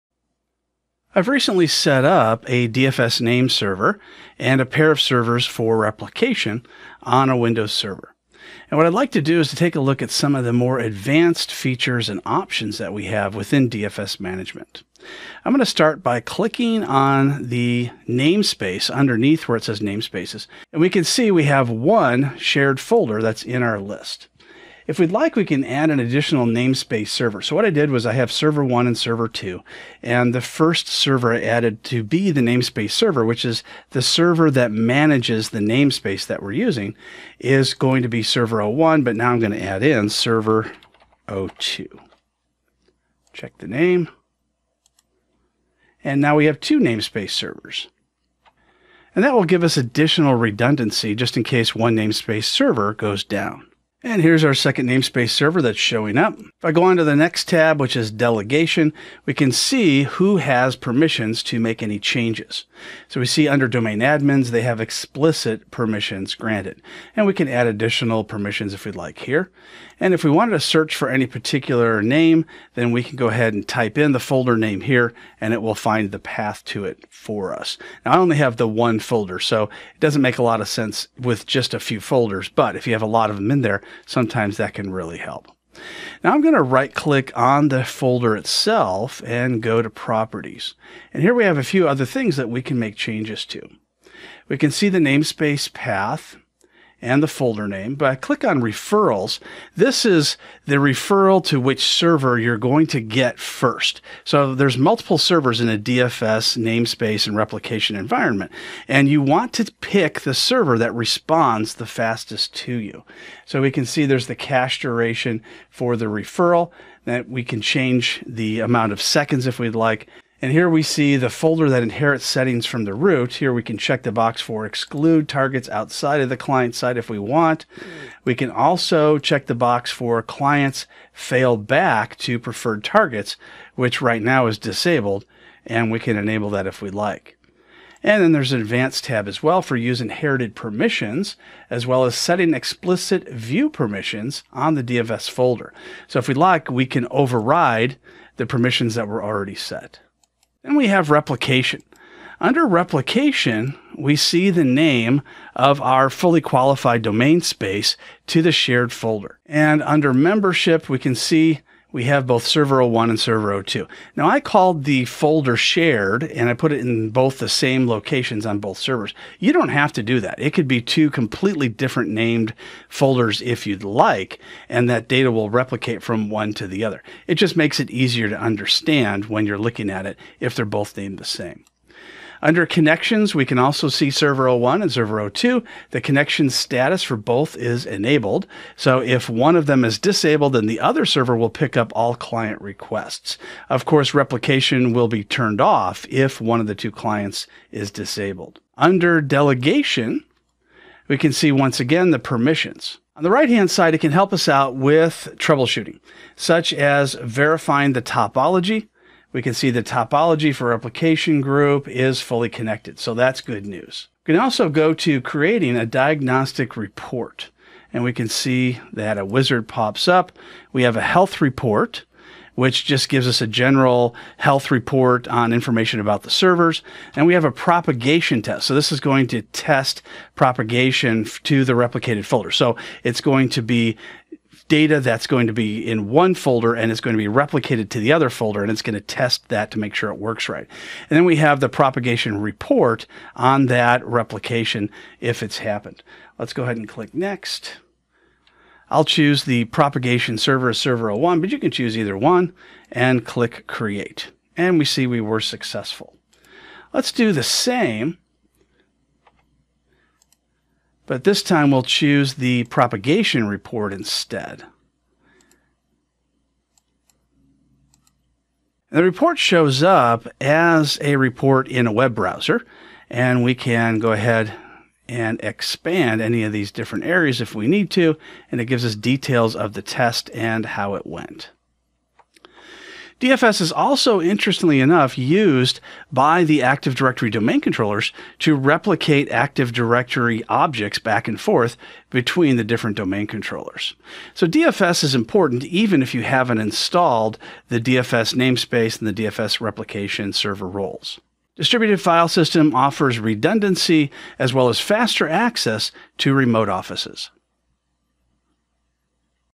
Advanced tutorial on Distributed File Systems on a Windows Server
Professor Robert McMillen shows you an advanced tutorial on Distributed File Systems on a Windows Server including many of the settings and features not usually shown.